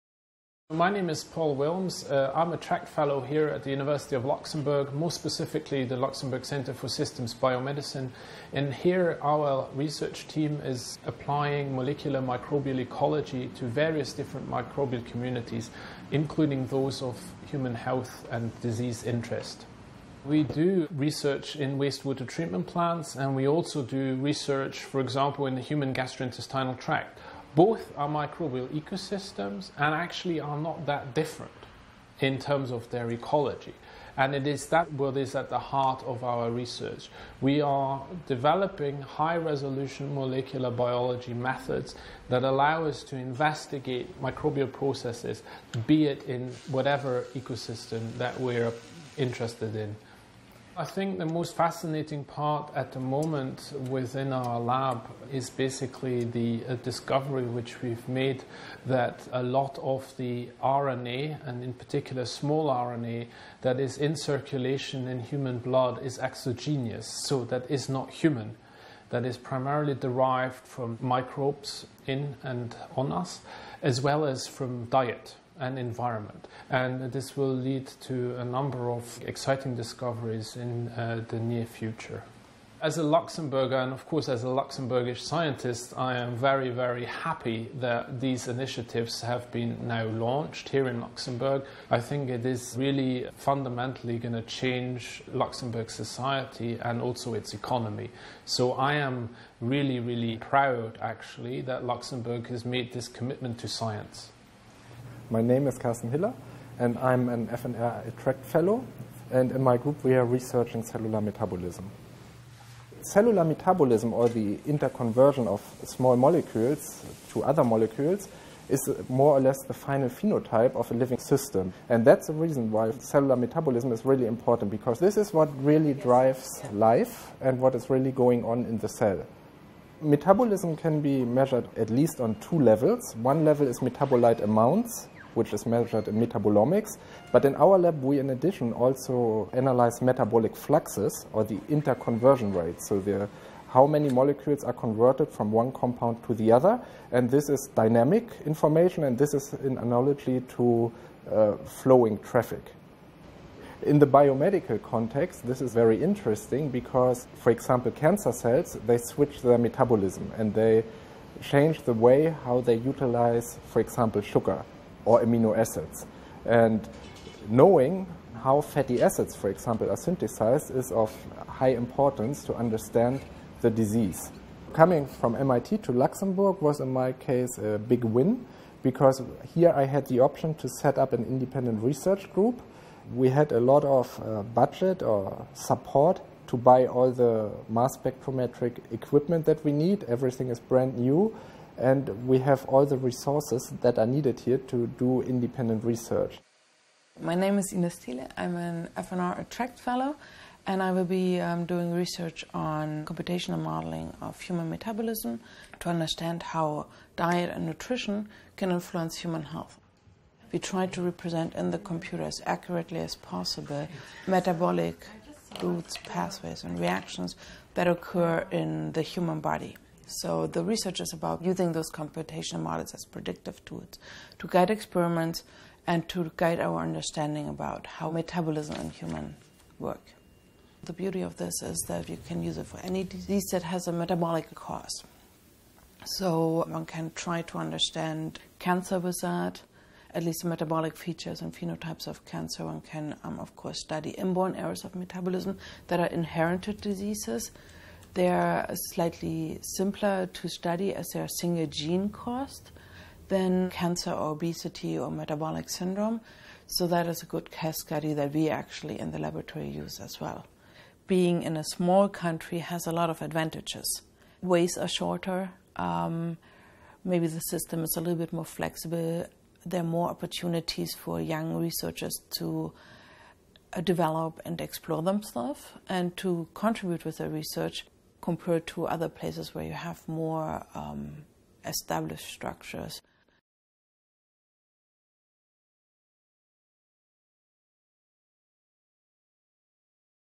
FNR ATTRACT Fellows in Biomedicine
This short film gives an insight into the FNR ATTRACT Fellows' research: Paul Wilmes, Karsten Hiller and Ines Thiele, researchers at the LCSB (Luxembourg Centre for Systems Biomedicine, University of Luxembourg), talk about their research projects in biomedicine which are funded by the Fonds National de la Recherche